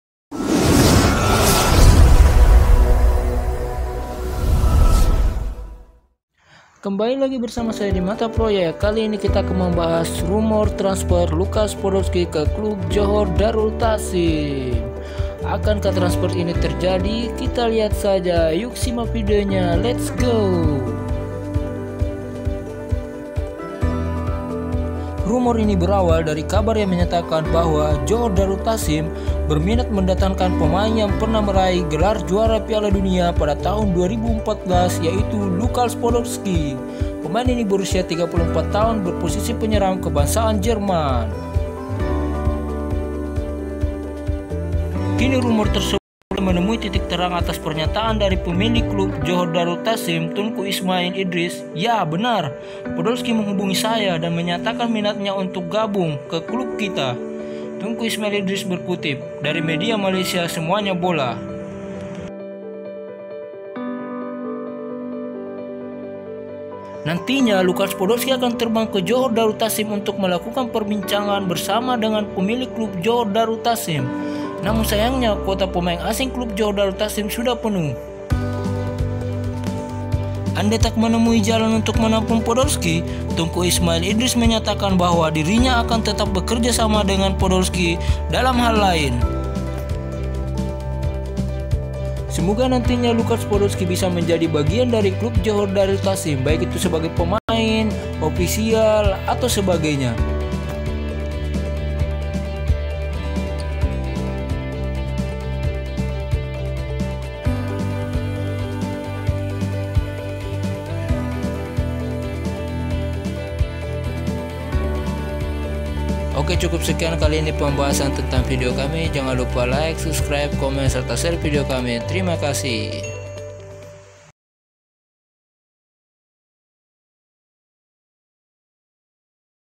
Info resmi !! Lukas Podolski Akan Gabung dengan JDT pemilik Stadion Sultan Ibrahim Larkin
Lukas Podolski akan gabung ke Klub JDT ,akankah transfer ini terwuju,langsung saja simak vidionya.

sumber:seumanyabola,google